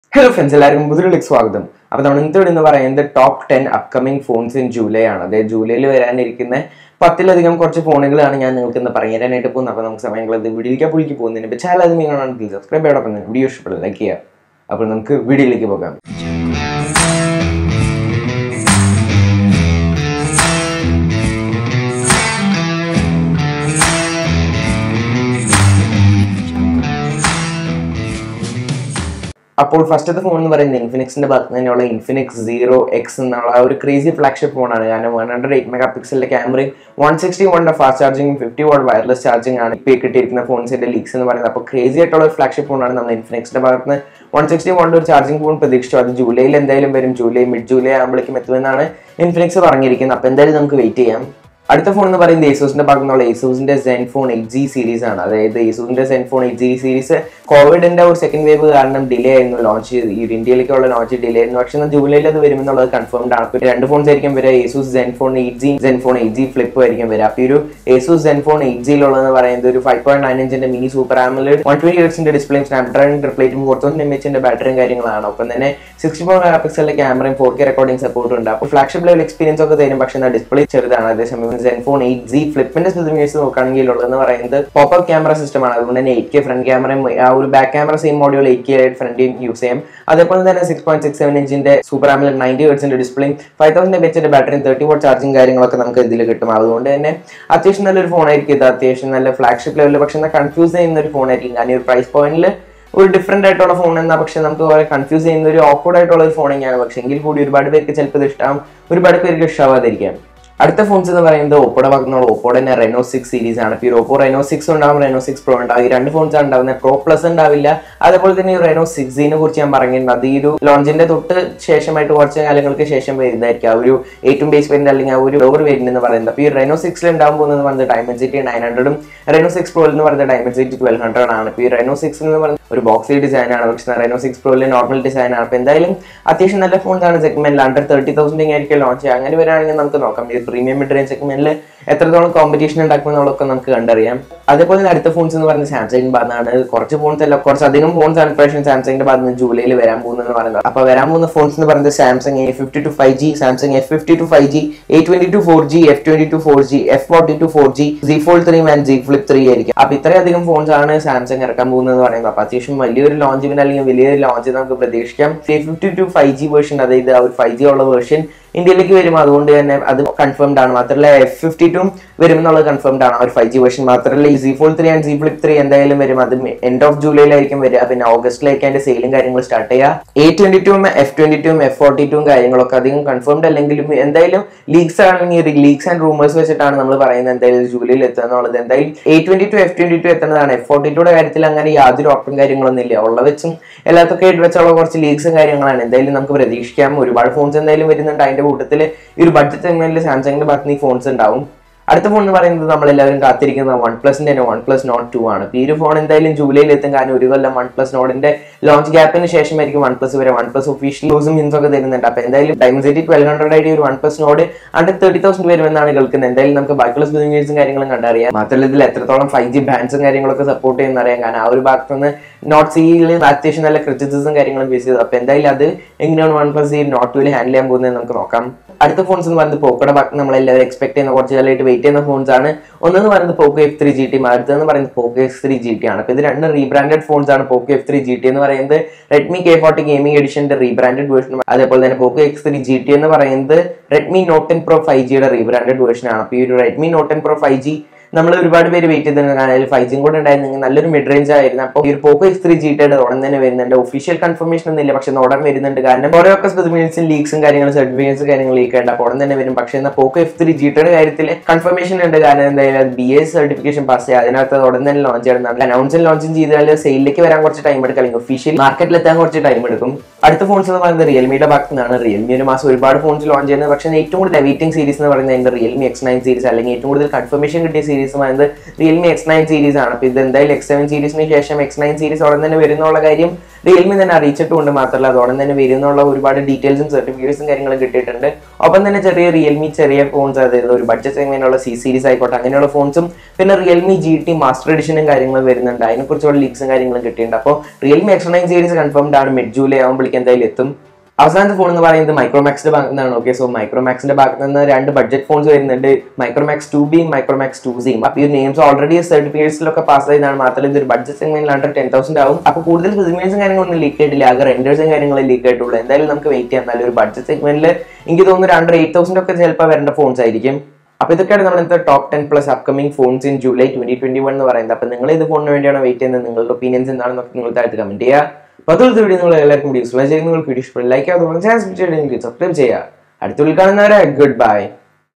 TOP 10+ Upcoming Phones in July 2021 | Samsung,Oneplus,Xiaomi, Realme,Micromax,Oppo | Tech Pool
This video is about the TOP 10+ upcoming smartphones in July 2021 !! So hope you enjoyed the video and found it useful !!